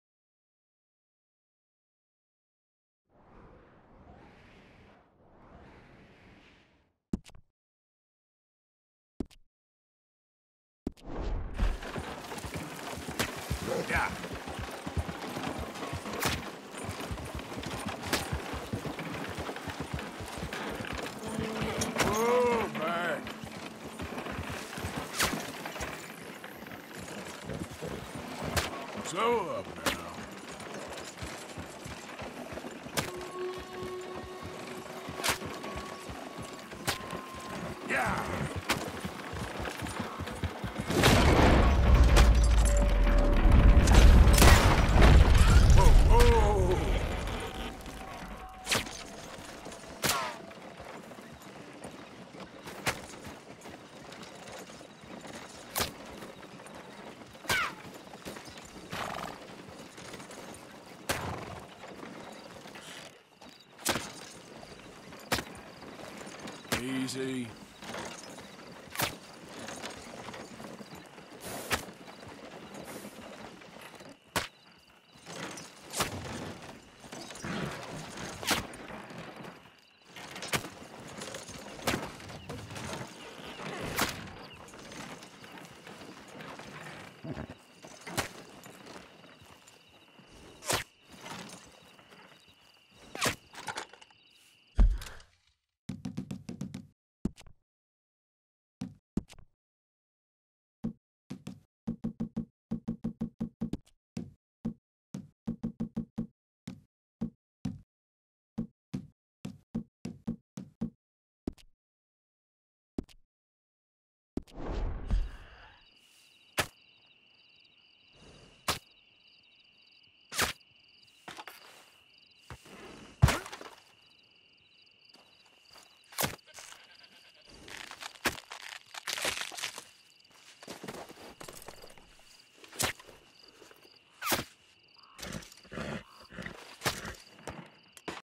HUNTING ARMADILLOS FOR ARTHUR THE BILLY VEST
Almost definite spawn location for armadillo. 90% of the time its 3* and 50% of the time there's 2. So you can have the Billy vest for Arthur. Gila Monsters can be hunted the same way.